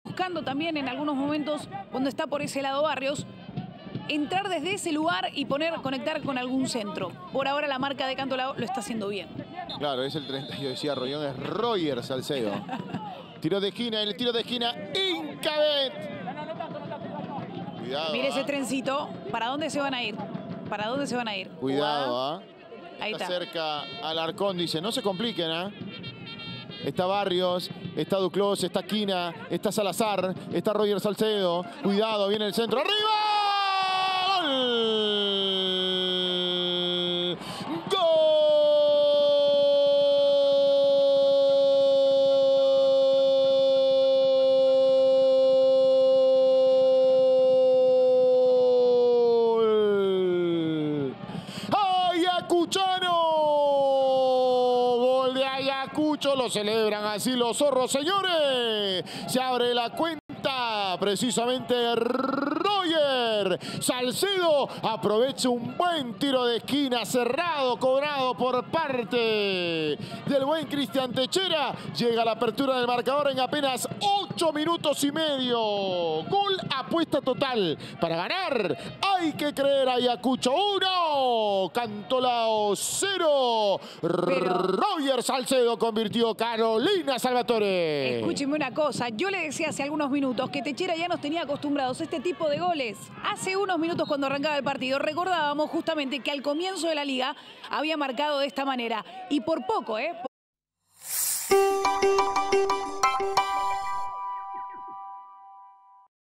Liga1 Betsson: Royer Salcedo anotó así el Ayacucho FC 1-0 Academia Cantolao
¡EL PRIMERO! Con un cabezazo 🔥, Royer Salcedo ⚽🇵🇪 anota el 1-0 a favor de Ayacucho FC ⚪🟠 ante Academia Cantolao 🟡⚫.